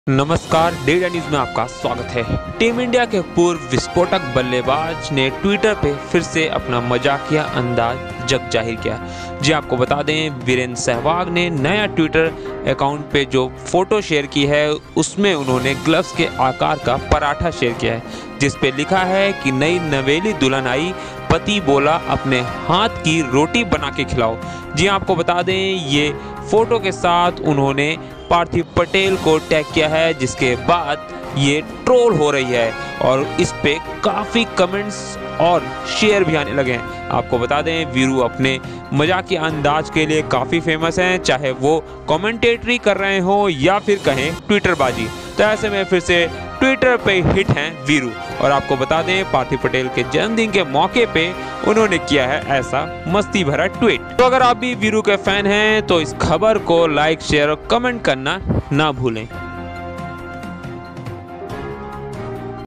VIRU का TWEET देशभऱ में मचा रहा है धमाल
Former Indian opener Virender Sehwag's escapades on Twitter are now the stuff of legends. The Twitter King enthralls his followers with some of the most interesting, funny and retweet-worthy tweets. Whether it's his naughty birthday messages or his skillful take downs